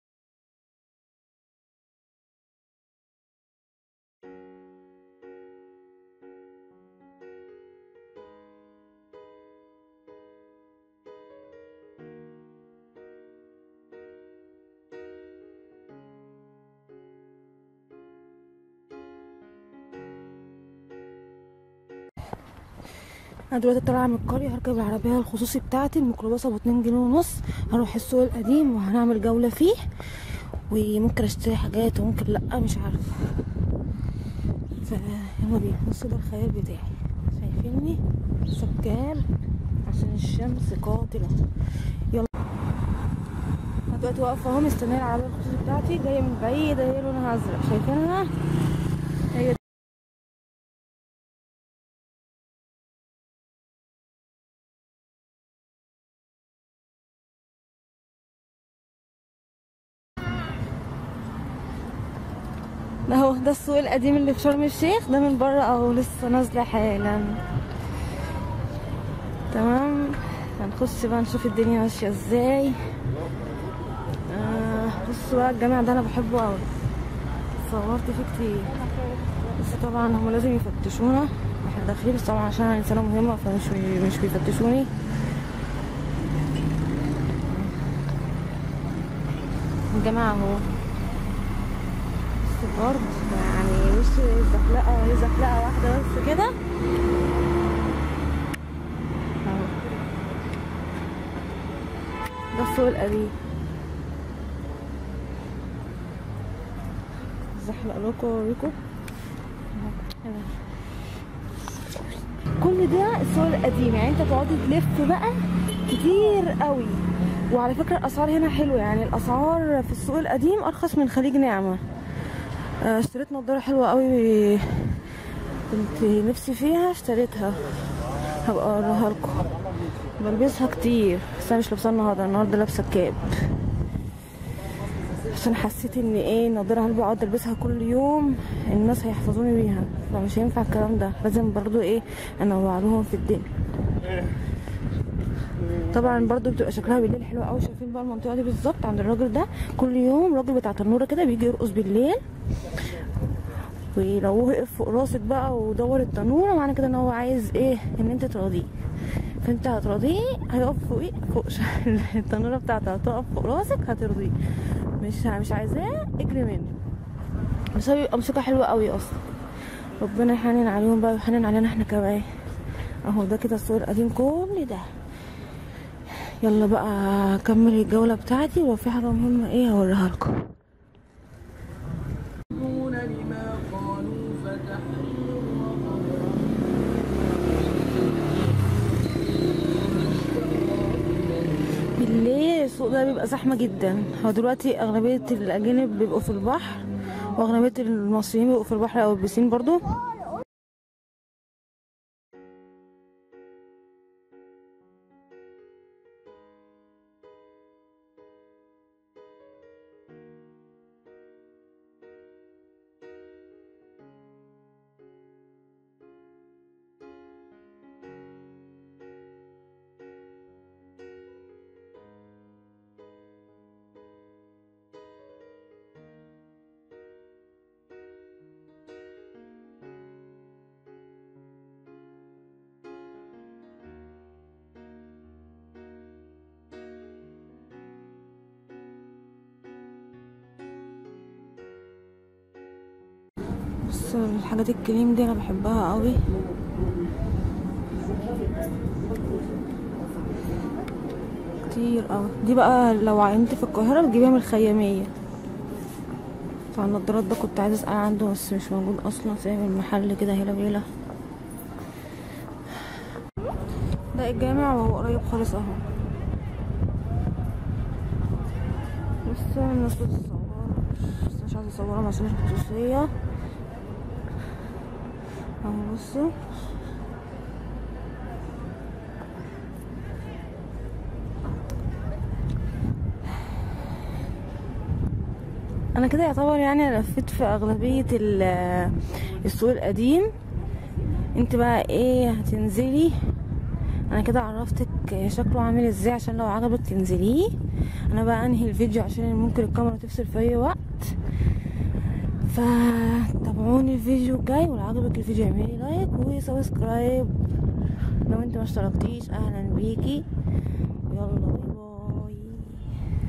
جولة فى السوق القديم شرم الشيخ| فطار على منظر مريح للاعصاب
الفيديو قبل اعلان الكورونا فى مصر . حبيت افرجكو على مصر والاماكن السياحيه  كمان .